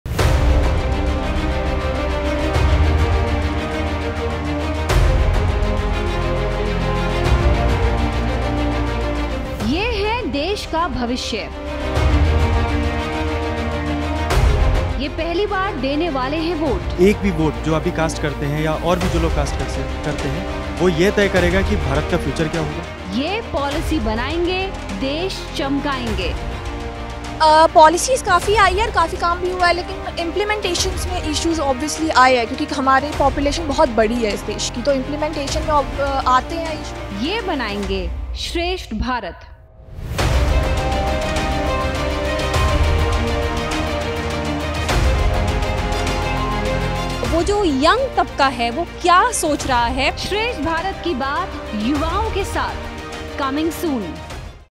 promo देश, समाज, Economy, Election और बदलाव पर क्या सोचता है युवा भारत? Special Series- Coming Soon
What does young India think about country, society, economy, elections and change? SPECIAL SERIES- COMING SOON